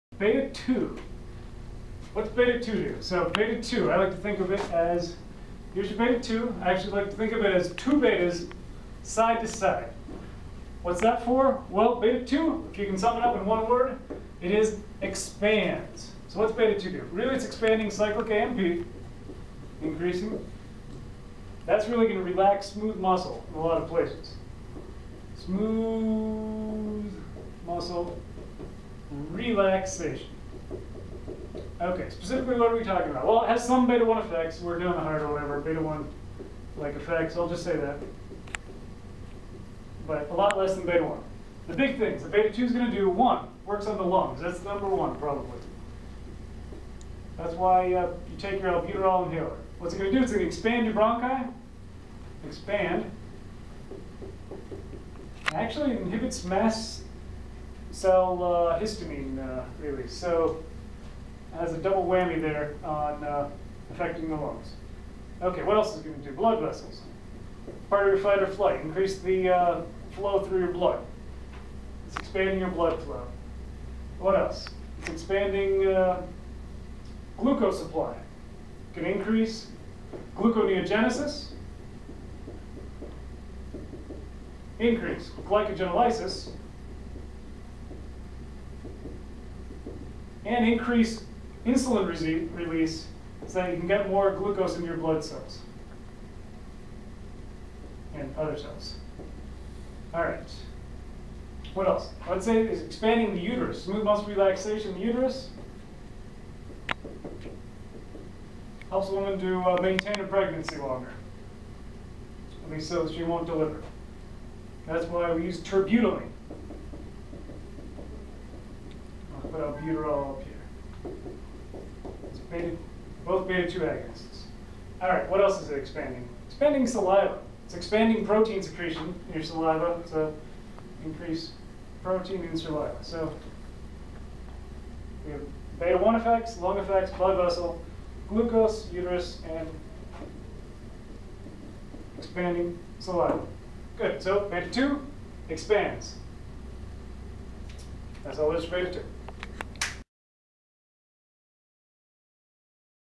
Beta2 Receptor HD
Beta2 Adrenergic Autonomic Receptor of the Sympathetic Nervous System presented in a pictorial mnemonic format . Key Point: Beta2 expands. For Medical learning.